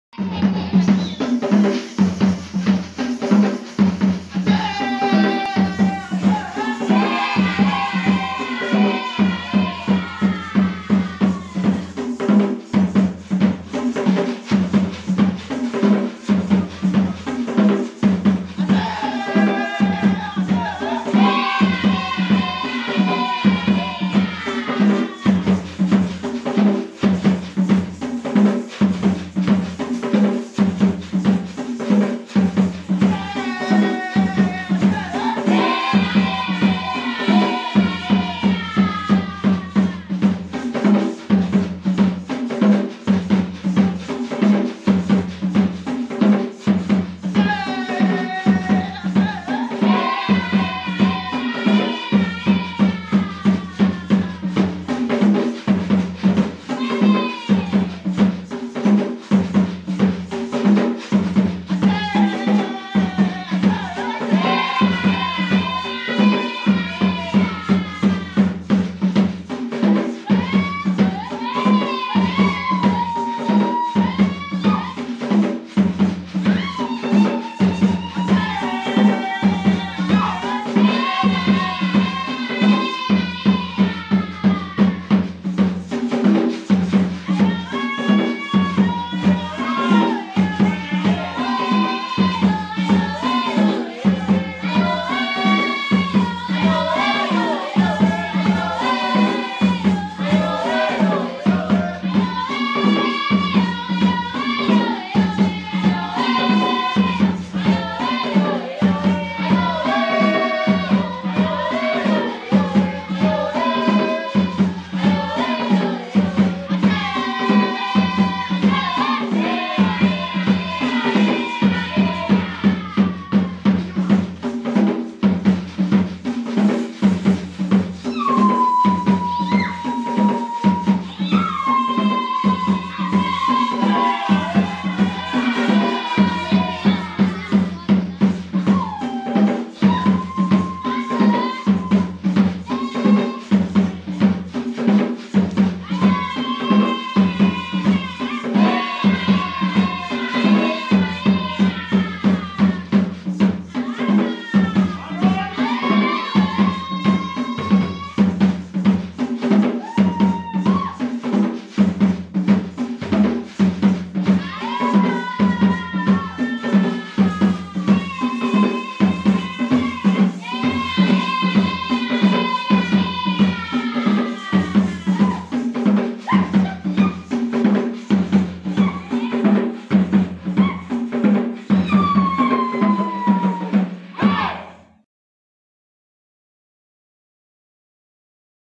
Karachuonyo Dance Of The Luo Community Found In Kenya
Hi guys In This video is a Traditional Dance called Karachuonyo is another Luo Culture Dance community. Dance by Men only while Singing.
Karachuonyo is originated in  Nyanza Province of Kenya. Its capital and largest town is Homa Bay.